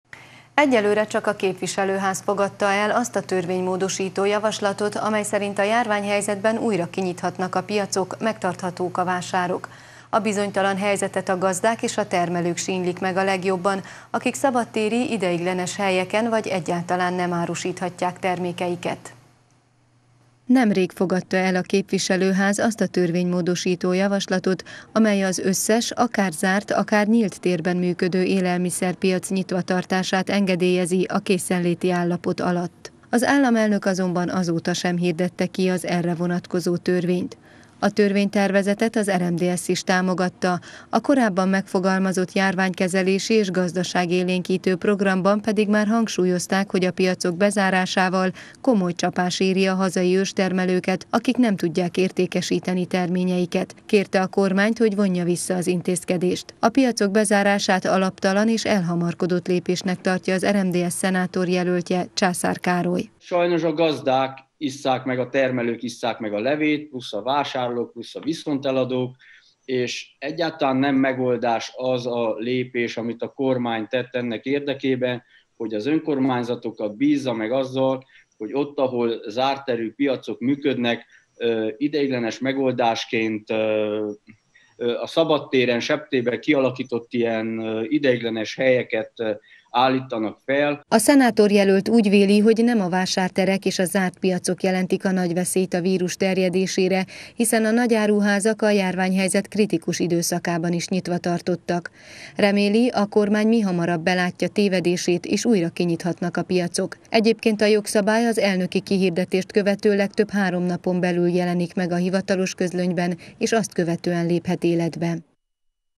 Piacok: a kormány kezében a döntés – Erdélyi Magyar Televízió
Egyelőre csak a képviselőház fogadta el azt a törvénymódosító javaslatot, amely szerint a járványhelyzetben újra kinyithatnak a piacok, vásárok. A bizonytalan helyzetet a gazdák, termelők sínylik meg a legjobban, akik szabadtéri ideiglenes helyeken vagy egyáltalán nem árusíthatják termékeiket.